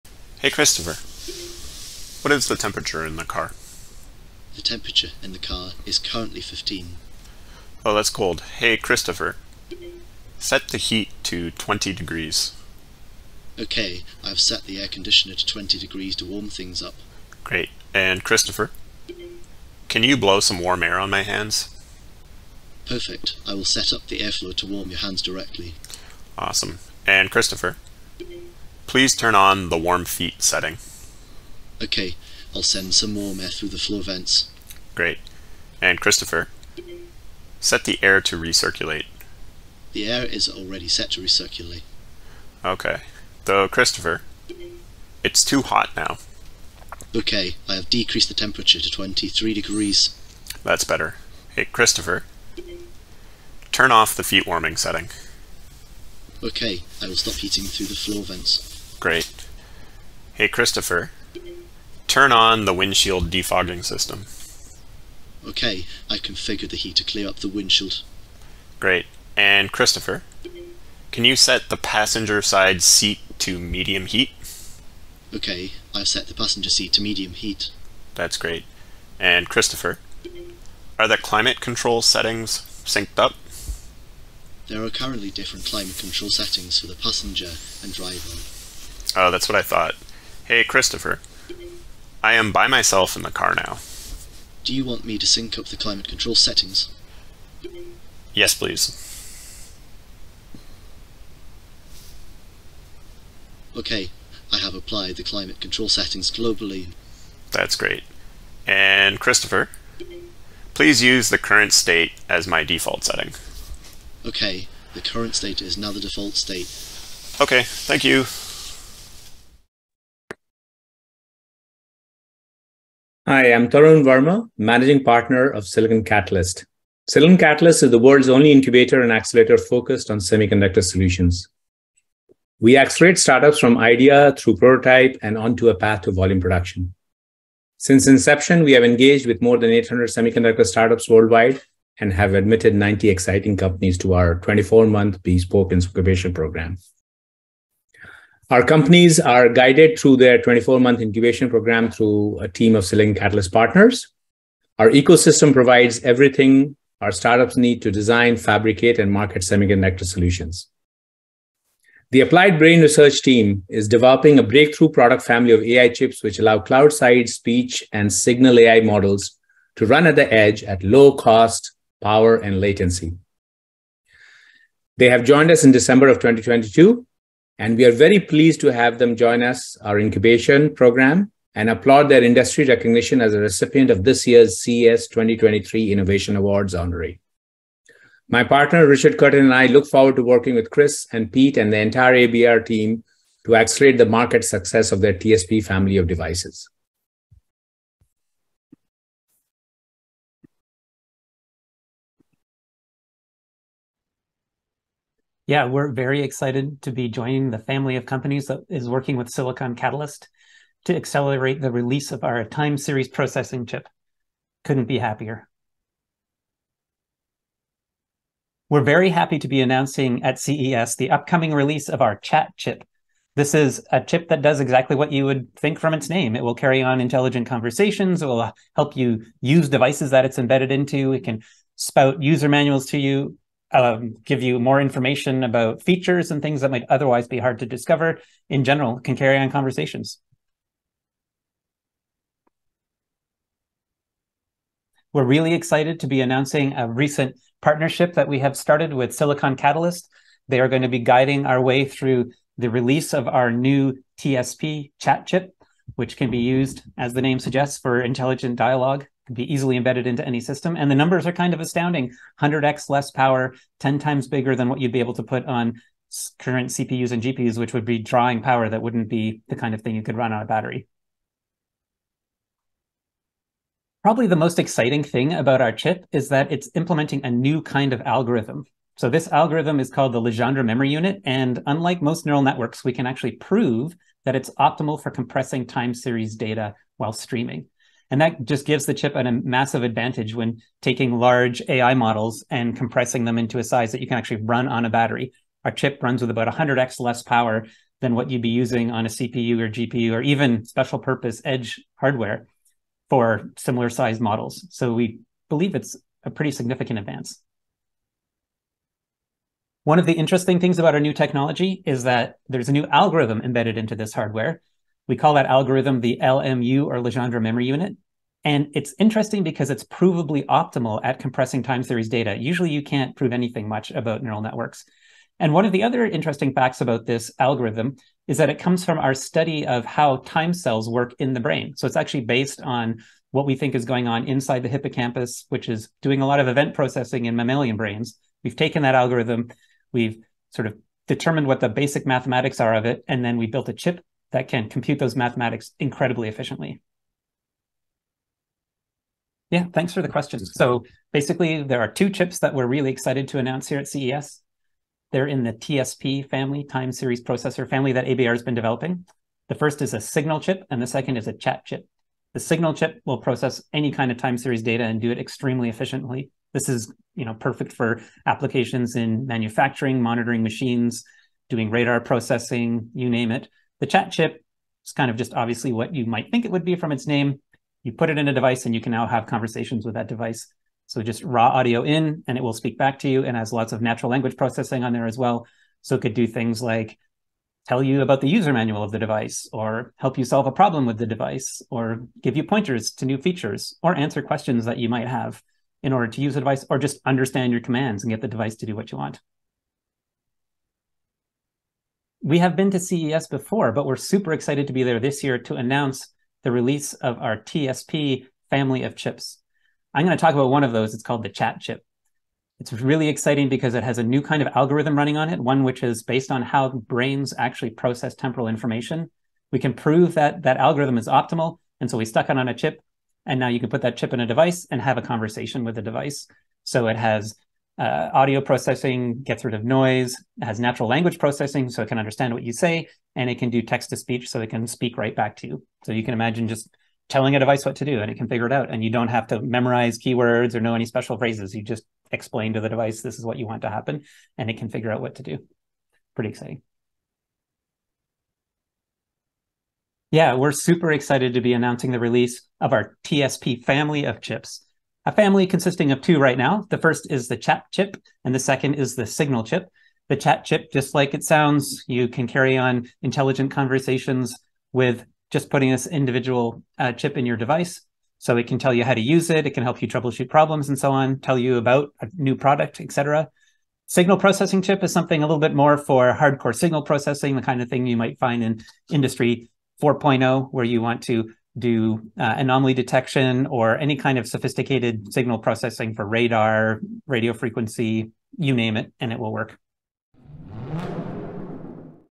Applied Brain Research Future AI Car Dashboard Display at CES 2023
ABR's Time-Series-AI Processor (TSP) - Cloud-sized AI on a small chip that goes anywhere delivering Alexa-On-A-Chip-like functionality, and Cloud-Sized Signal AI Processing.